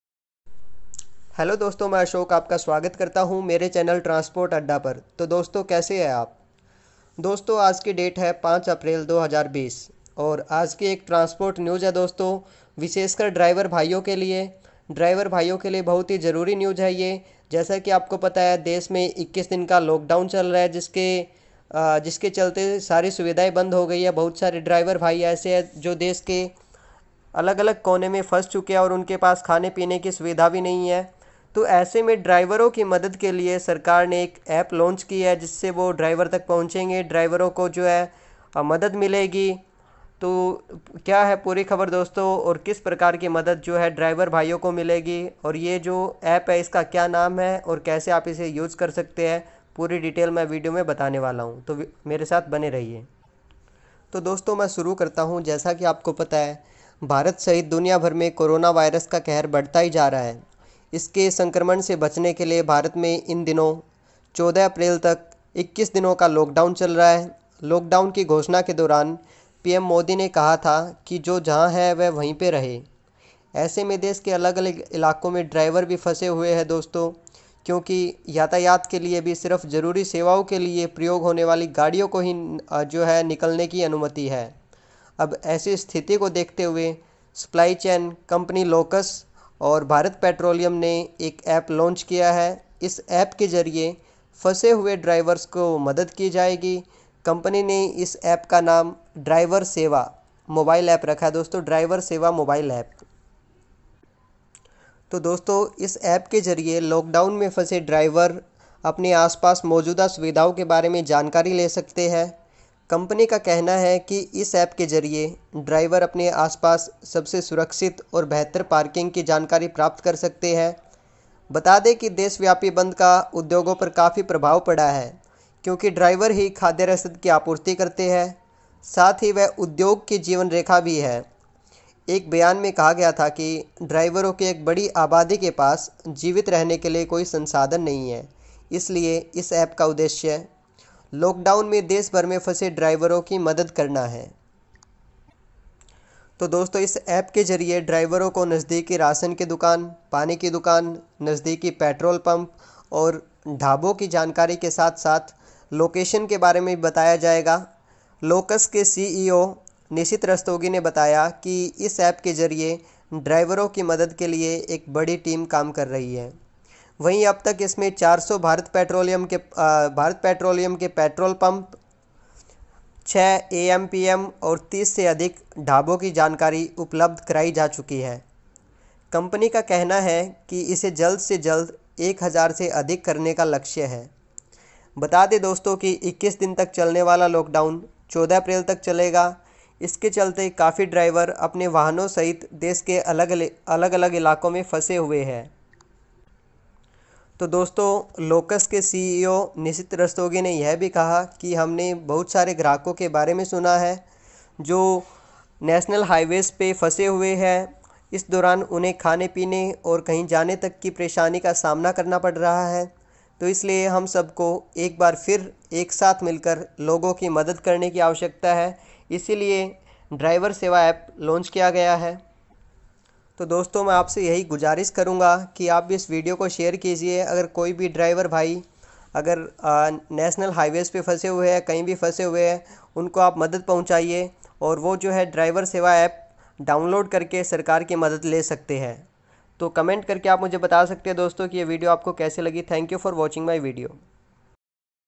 ड्राइवर सेवा मोबाइल ऐप - लोक डाउन में फंसे ड्राइवरों का सहारा ।
Supply chain optimisation platform Locus on Friday said it has launched a 'Driver Seva Mobile App' to help drivers stranded during the lockdown on account of the COVID-19 outbreak.

The app has been supported by Bharat Petroleum Corp Ltd (BPCL) and All India Motor Transport Congress (AIMTC), an apex body of transporters that has been raising the issue of stranded drivers.